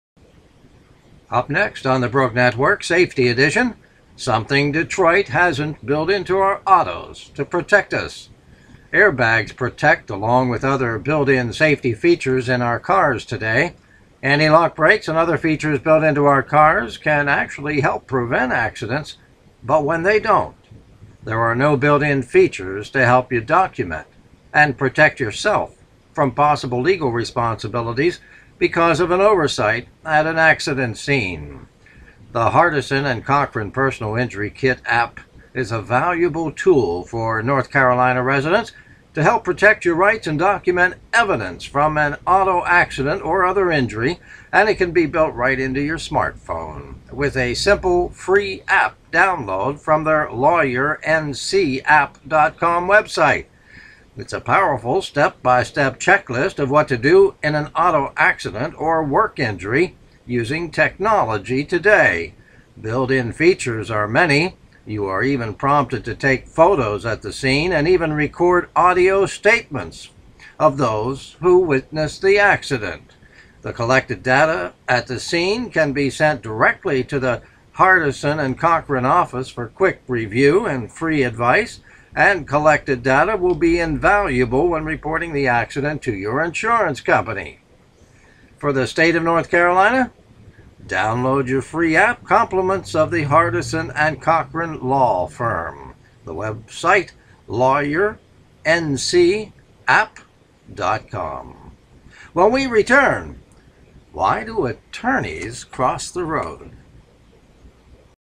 Hardison & Cochran Attorneys provide Smart Phone App for accident scene data.
Airbags to anti-lock brakes but no protection at the accident scene.  North Carolina residents can download a free smartphone app compliments Hardison & Chchran Attorneys.